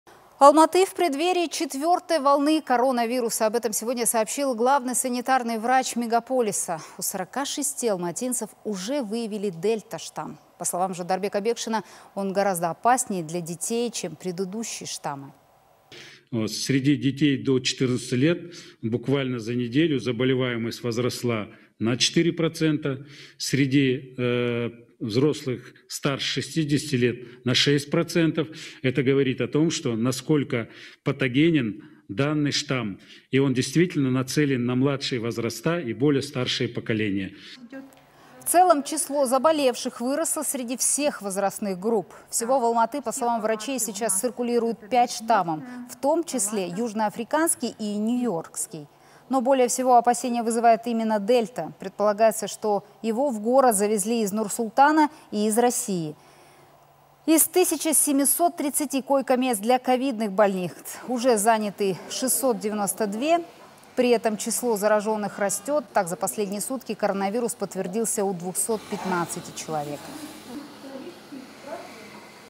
Алматы в преддверии четвертой волны коронавируса
Алматы в преддверии четвертой волны коронавируса. Об этом сегодня сообщил главный санитарный врач мегаполиса. У 46 алматинцев уже выявили дельта-штамм. По словам Жандарбека Бекшина, он гораздо опаснее для детей, чем предыдущие штаммы.
ЖАНДАРБЕК БЕКШИН, главный государственный санитарный врач Алматы:
– Среди детей до 14 лет буквально за неделю заболеваемость возросла на 14 процентов. Среди взрослых старше 60 лет – на 6%. Это говорит, насколько патогенен данный штамм, и он действительно нацелен на детей младшего возраста и более старшее поколение.

В целом, число заболевших выросло среди всех возрастных групп. Всего в Алматы, по словам врачей, сейчас циркулируют пять штаммов, в том числе южноафриканский и нью-йоркский. Но более всего опасения вызывает именно дельта. Предполагается, что его в город завезли из Нур-Султана и России. Из 1730 койко-мест для ковидных больных уже заняты 692. При этом число зараженных растет. Так, за последние сутки коронавирус подтвердился у 215 человек.